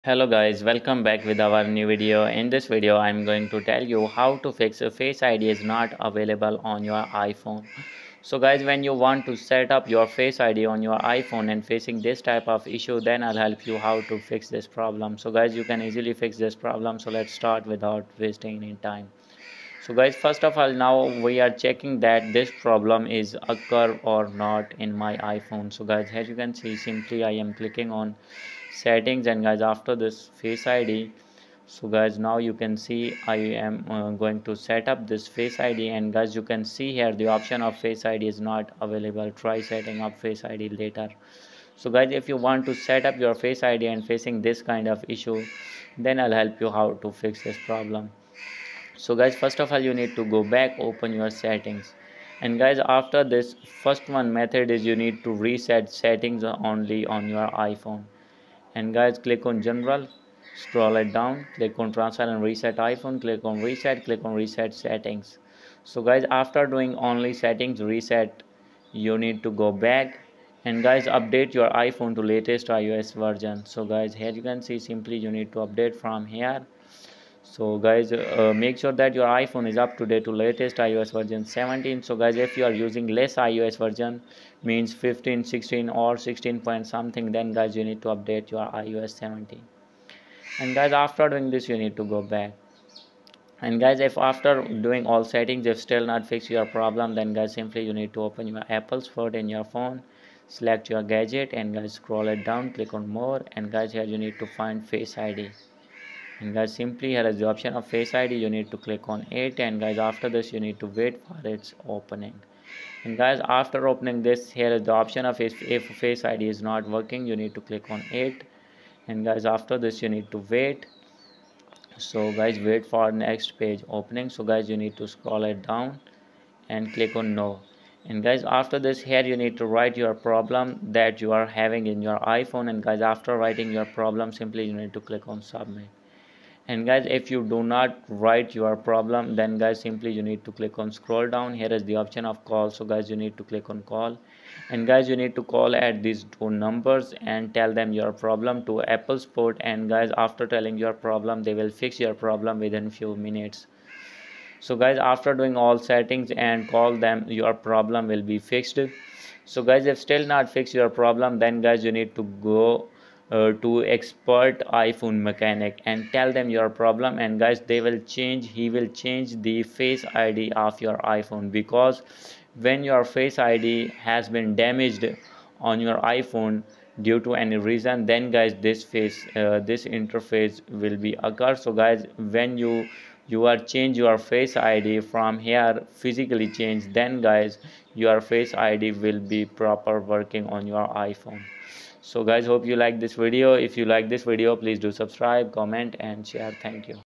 Fix: Face ID Is Not Available Try Setting Up Face ID Later On iPhone
In this video, I will help you to fix Face ID Is Not Available Try Setting Up Face ID Later On iPhone. Please watch till the end.